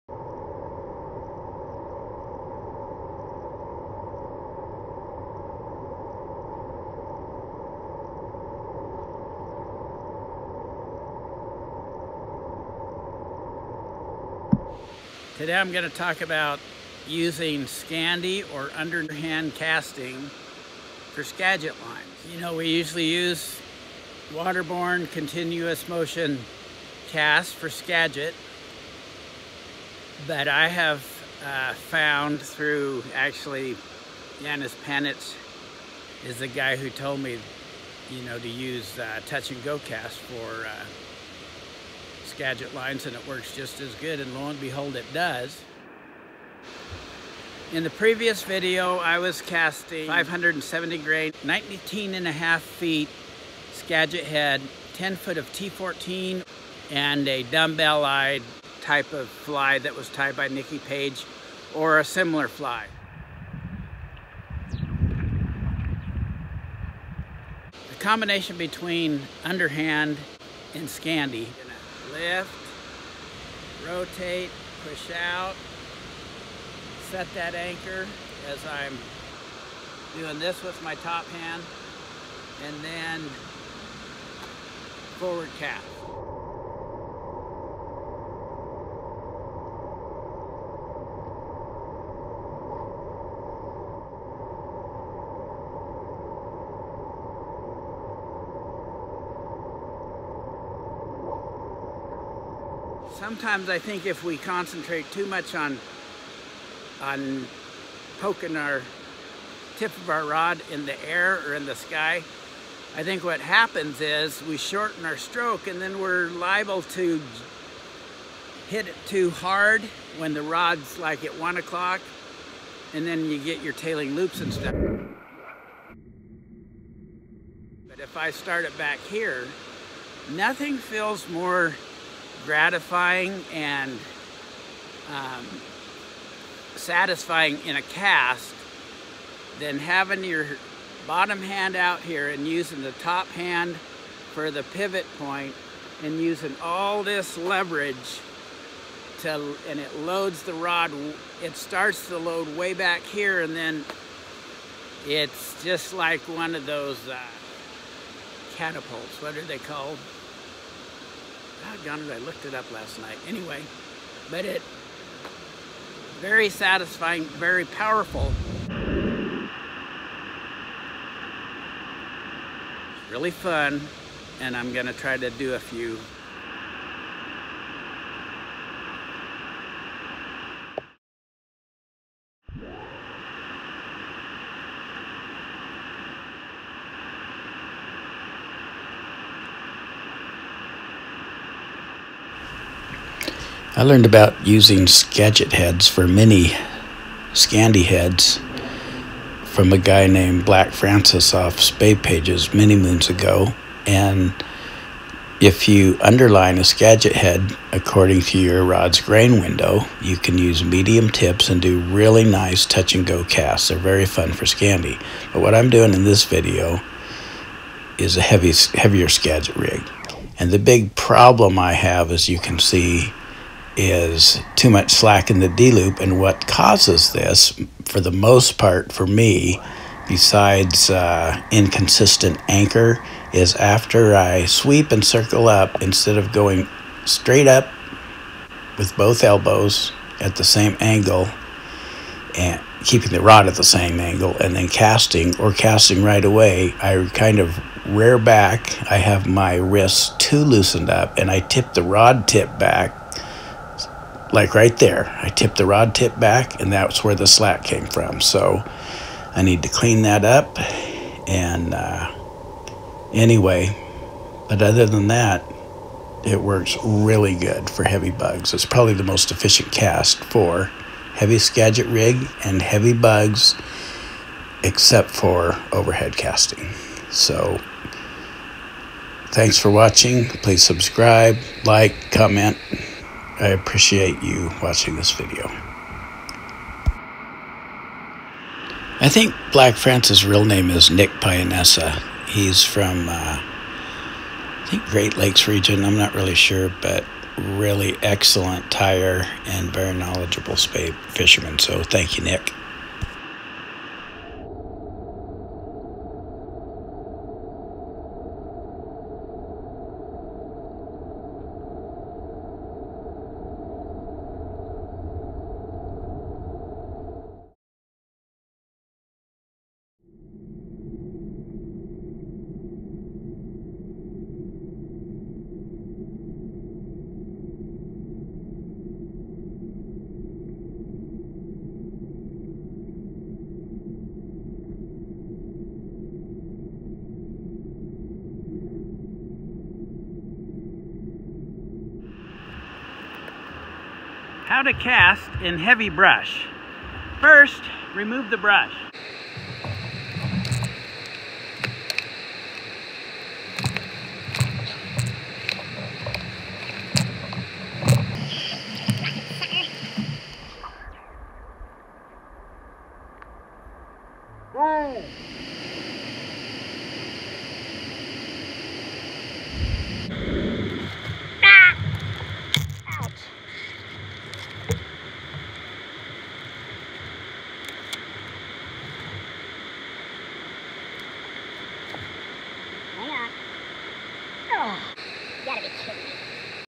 Touch And Go Skagit Casting, The Good, the Bad and the Ugly
Doing Splash and go casts with a Skagit setup